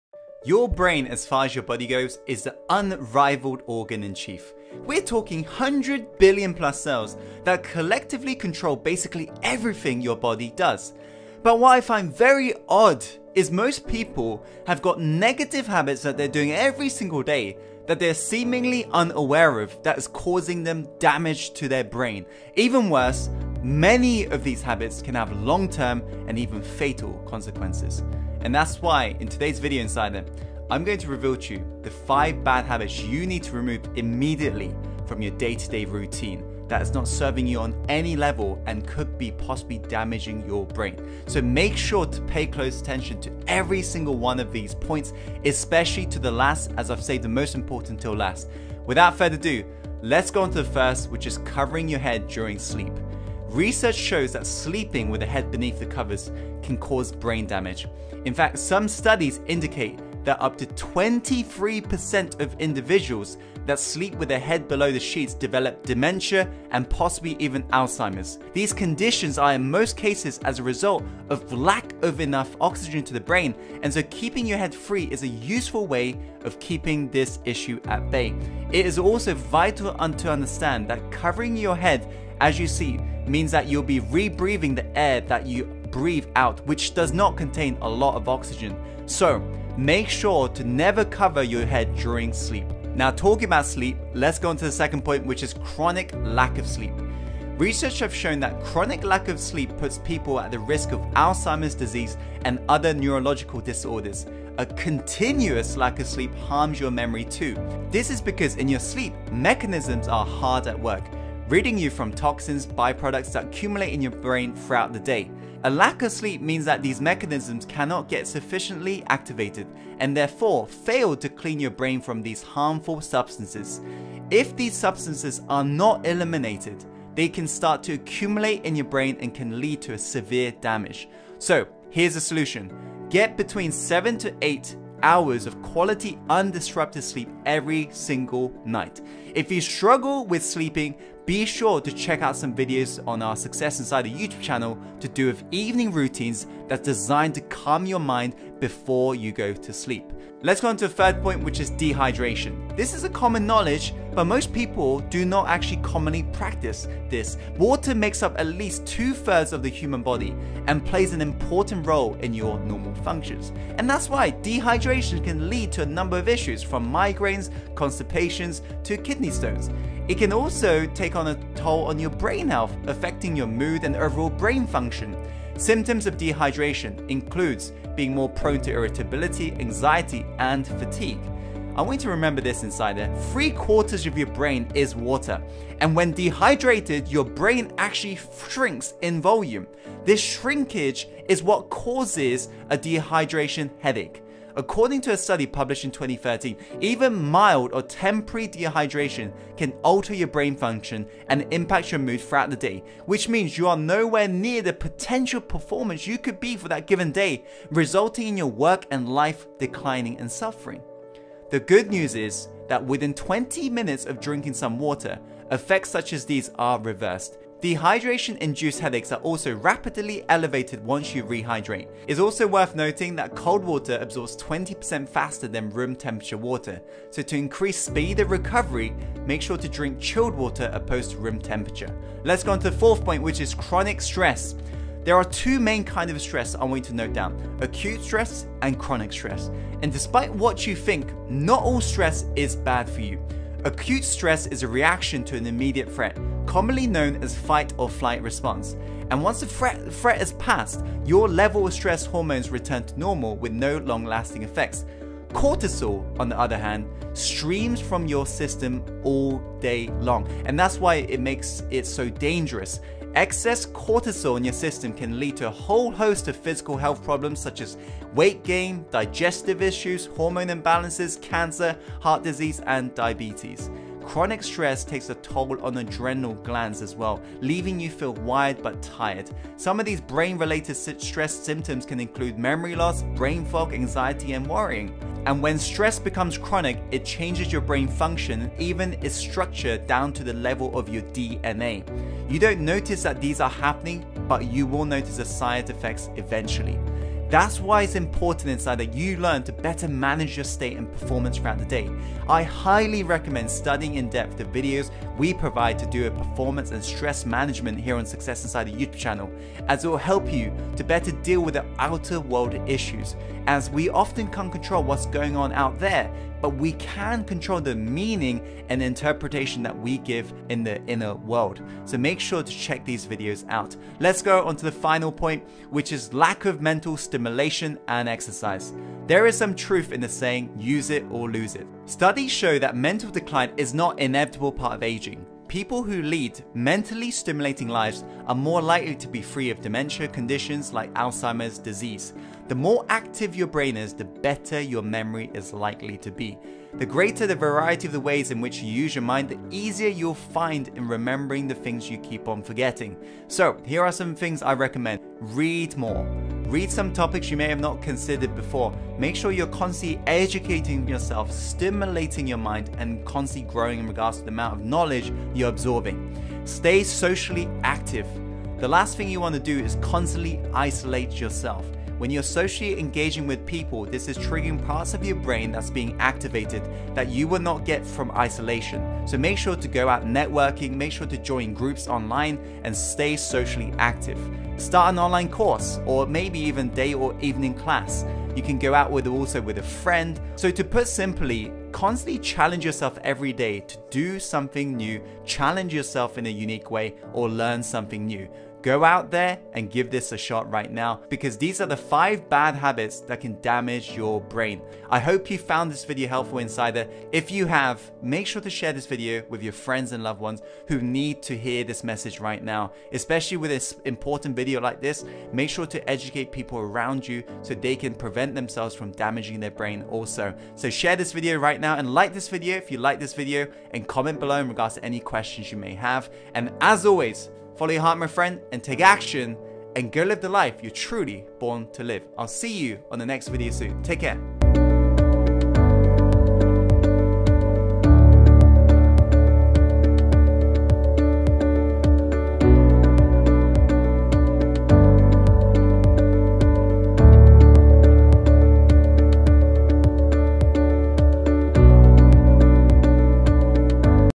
5 Bad Habits That Damage Your Brain
lack of sleep may be a cause of dementia, including Alzheimer's disease,  If you have trouble with sleep, avoid alcohol, caffeine, and electronics in the evening